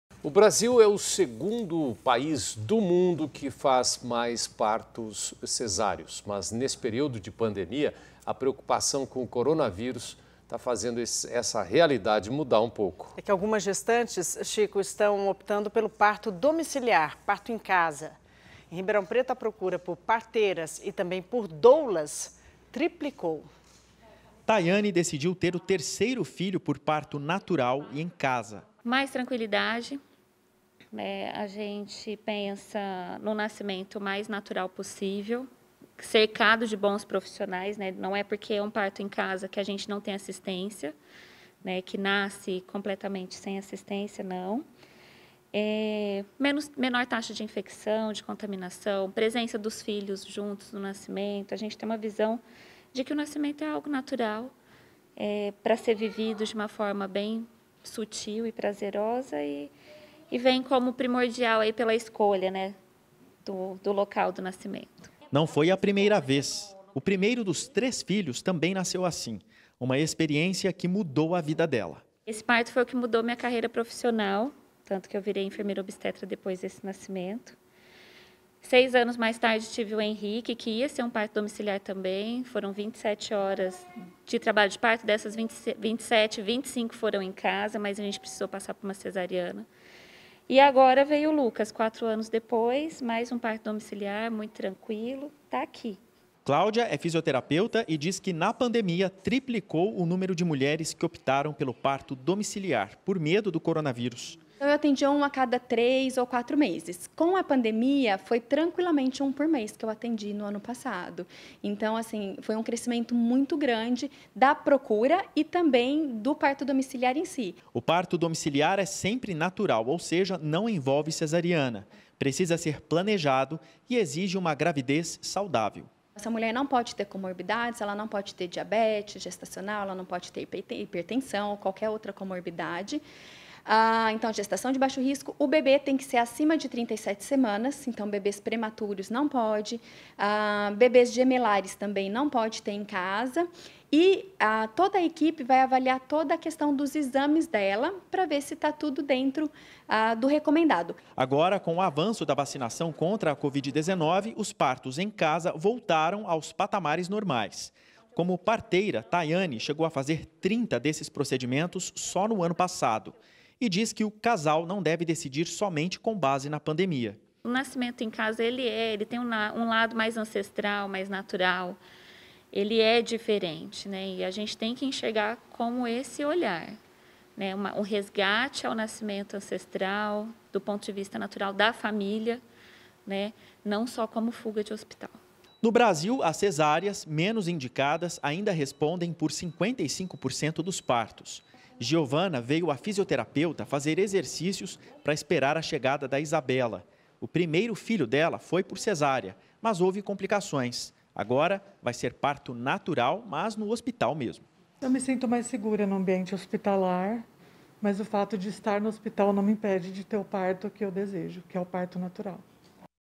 Aumenta procura por partos naturais na pandemia
O Brasil é o segundo país no mundo que mais faz cesáreas. Mas, neste período de pandemia, a preocupação com o coronavírus está fazendo essa realidade mudar um pouco. Algumas gestantes estão optando pelo parto domiciliar. Em Ribeirão Preto, a procura por parteiras e doulas triplicou.

Vem pro nosso canal no Telegram! Acesse